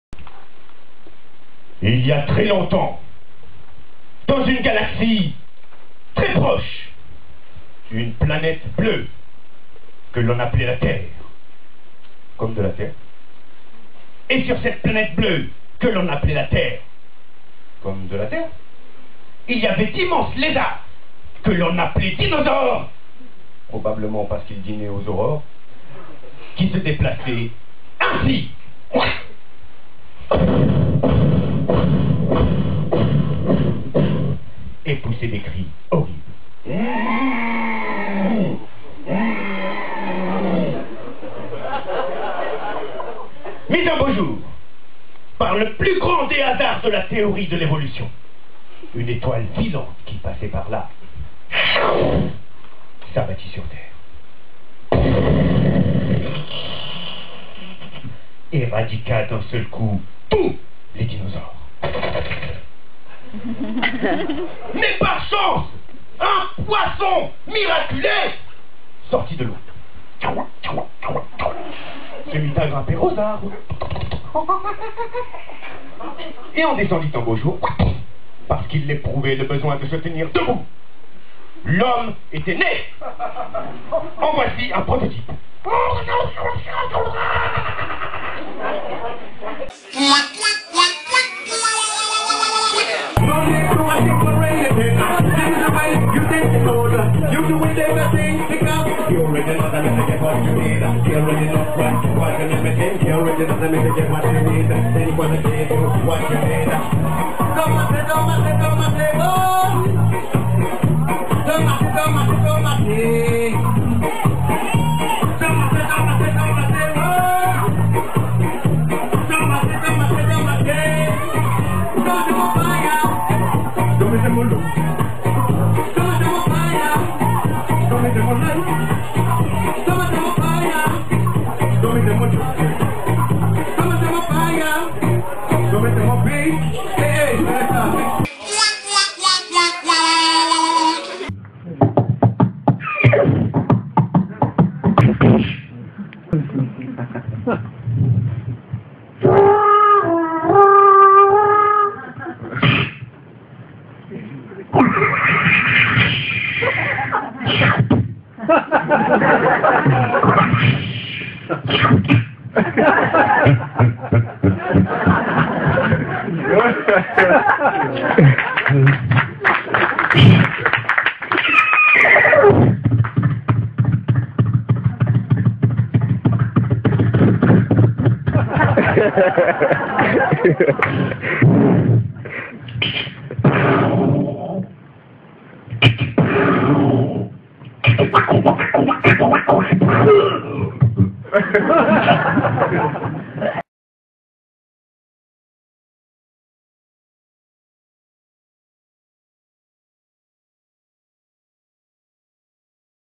David Bens, artiste multifonctions 4.
David Bens, artiste multifonctions vous présente son spectacle "tout en un" : Un one-rastaman-show basé sur le bruitage vocal, l'humour, le beatbox, le reggae et le slam.